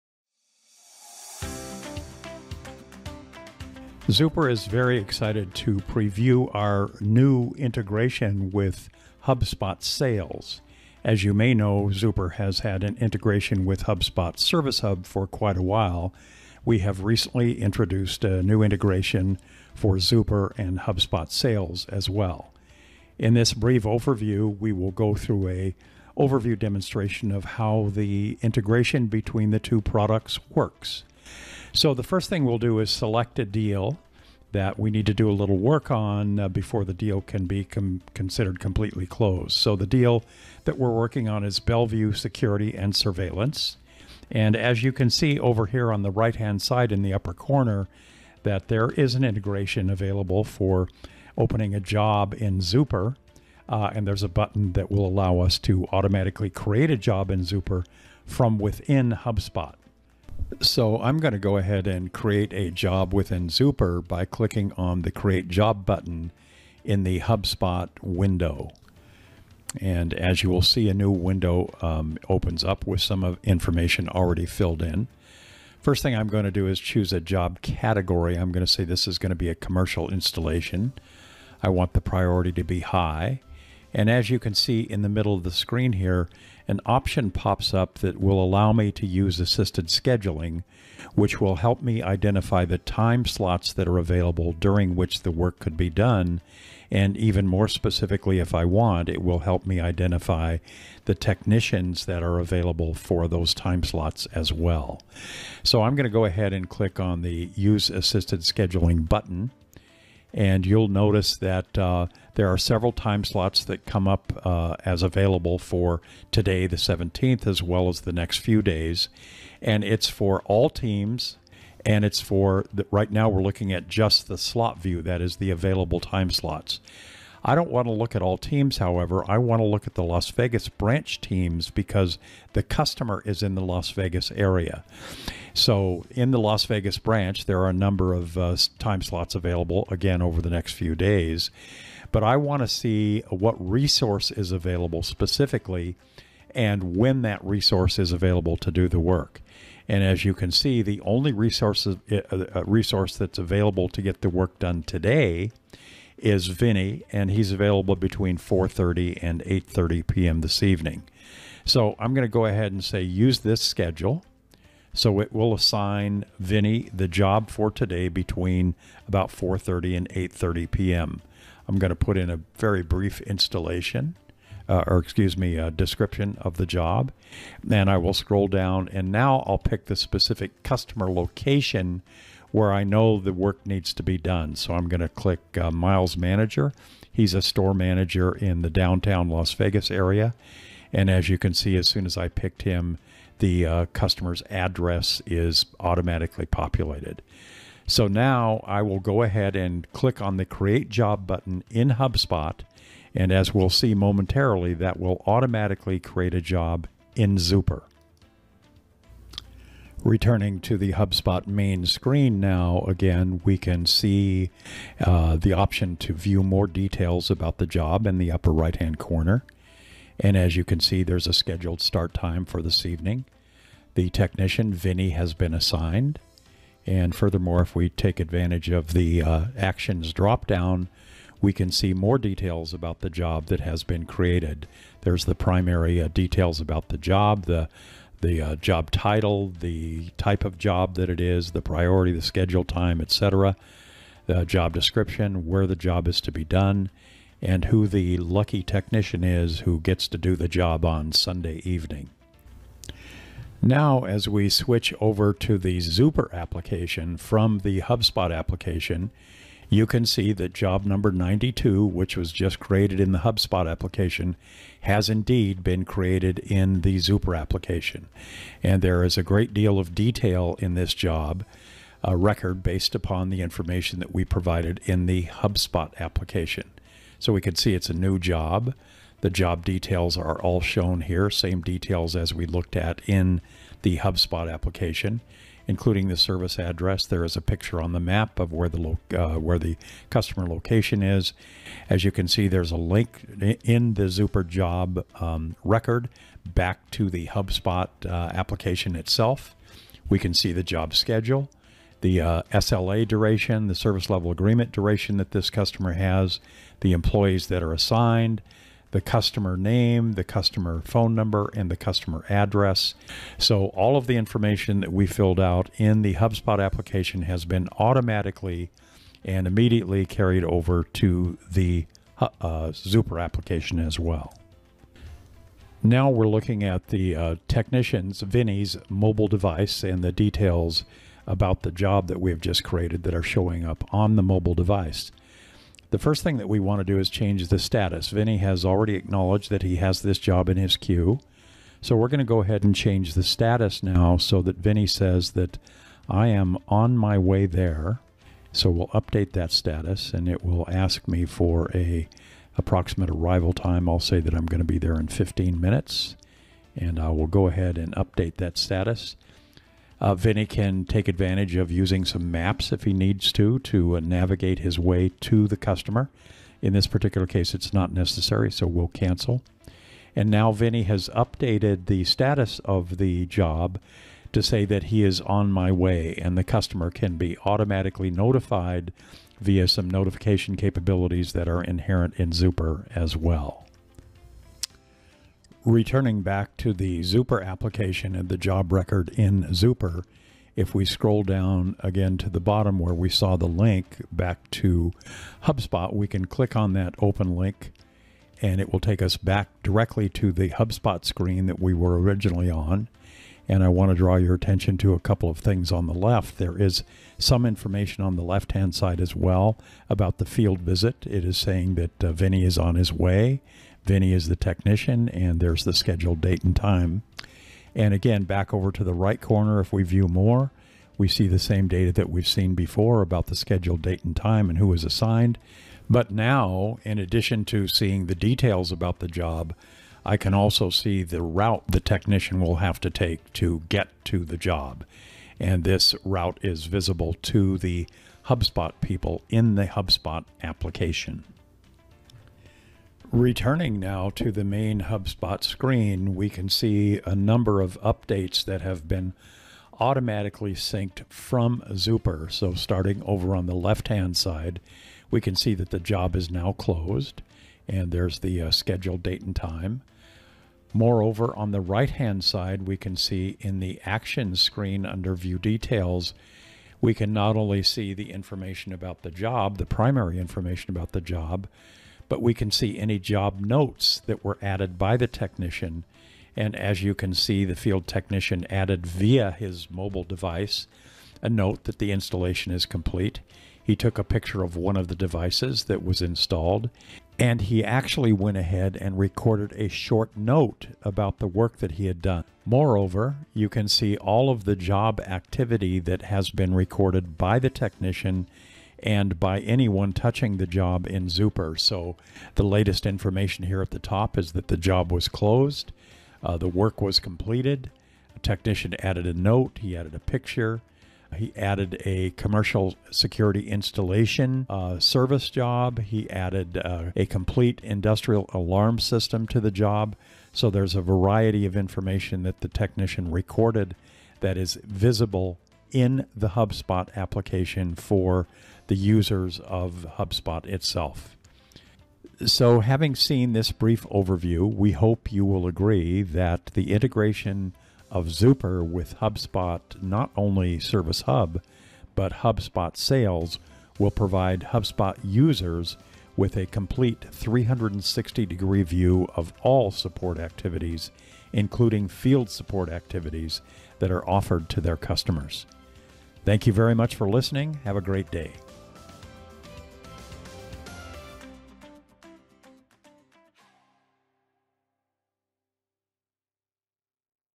How the Zuper and HubSpot Sales Hub Integration works?
In order to make the Zuper and HubSpot work seamlessly for you, we added an integration in the sales module that will allow you to directly create jobs in Zuper through HubSpot and a lot more. Watch the video to find out.

Book a trial with us today and organize your field service operations.